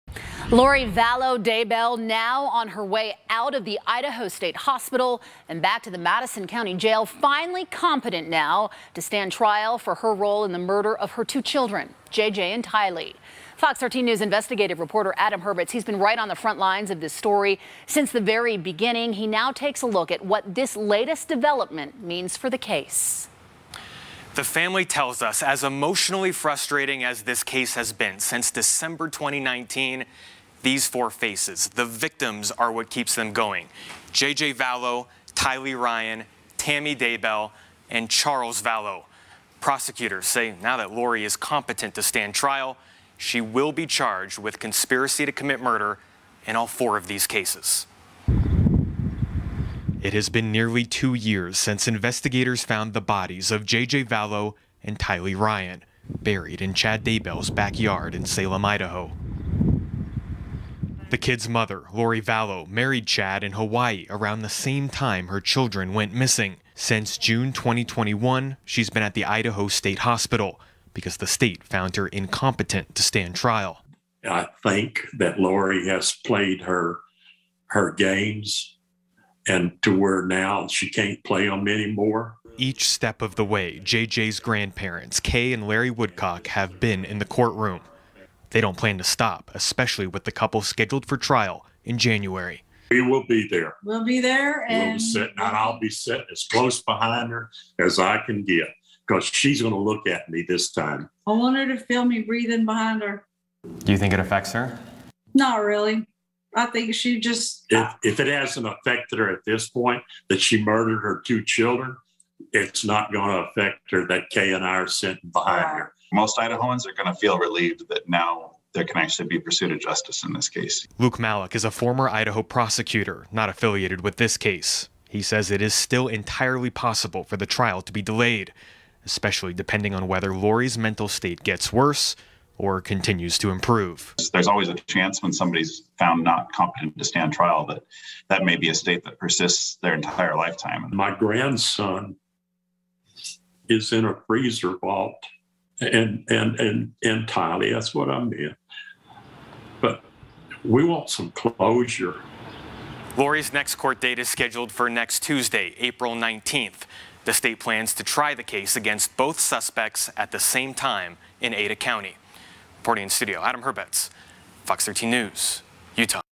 Court finds Lori Vallow Daybell fit for trial
Lori Vallow Daybell has been found fit to stand trial and will be arraigned for the murder of her children next week.